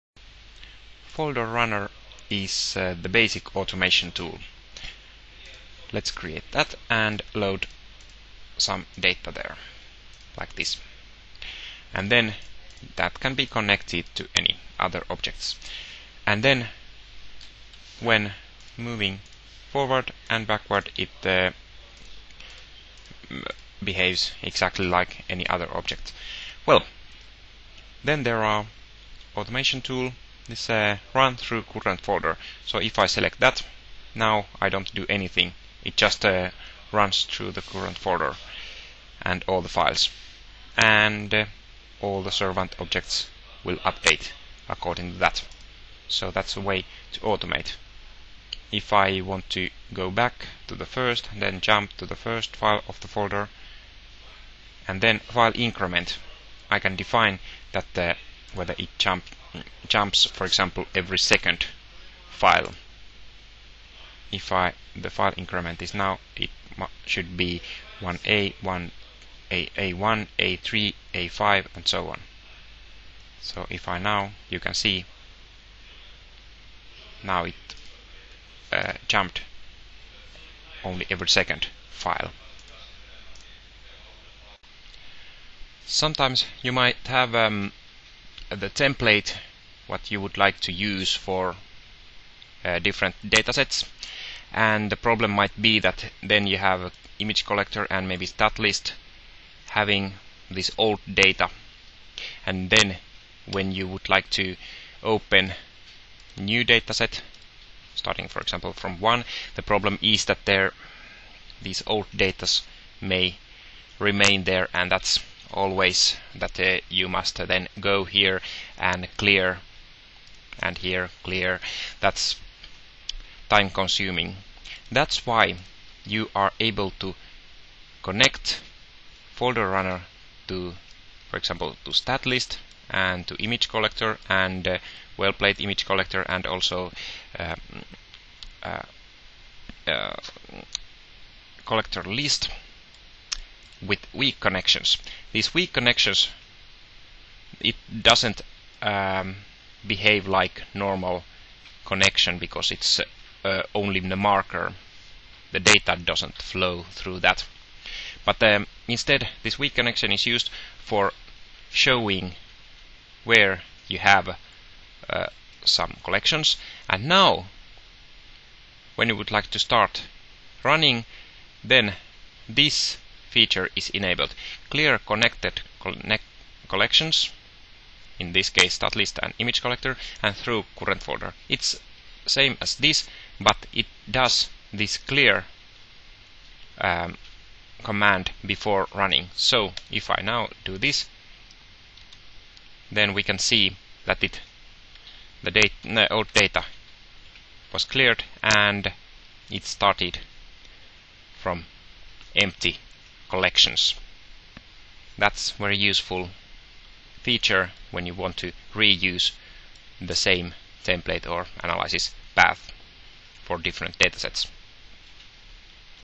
09. Folder runner
Folder runner is an automation tool which makes it possible to analyze a whole folder at once. File increment can be set. Together with statlist and/or image collector you will gain statistics and images from the whole folder with a one go.